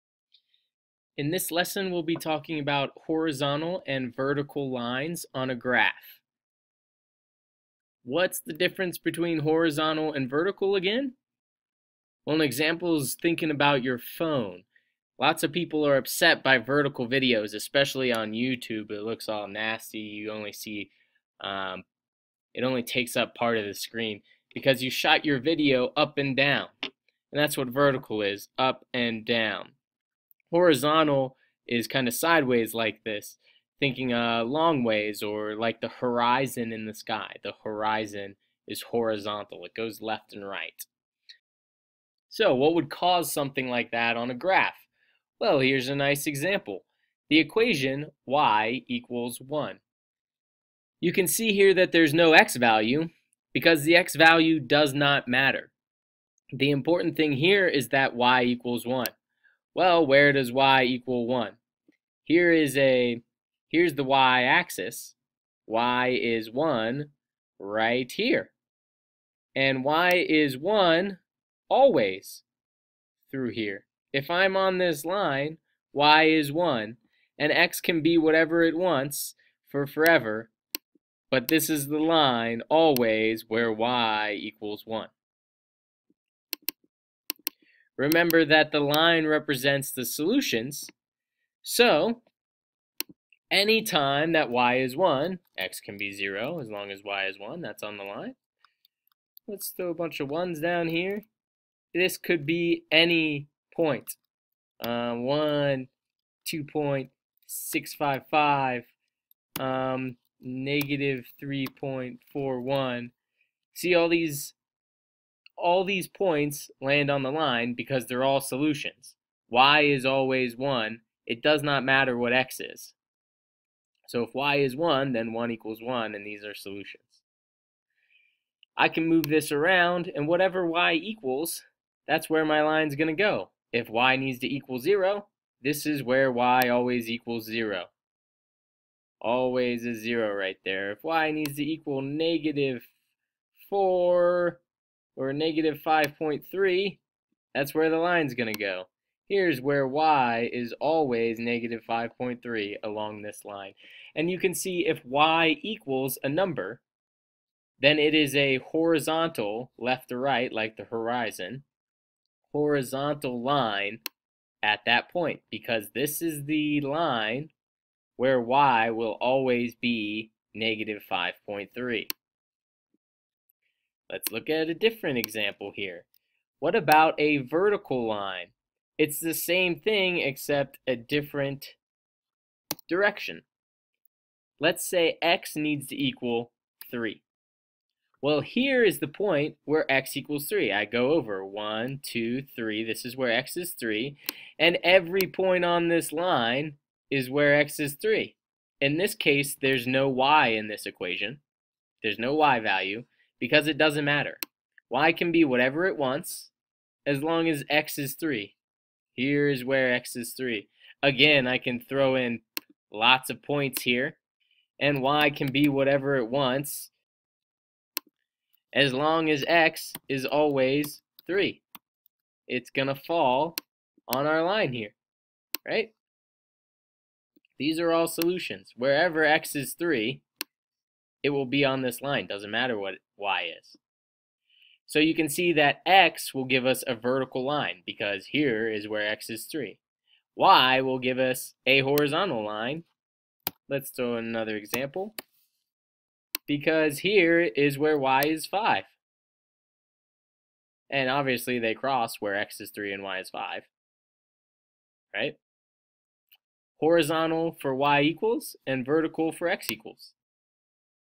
Horizontal and Vertical Lines on a Graph
Equations to Vertical and Horizontal lines on a graph. Why do they look the way they do?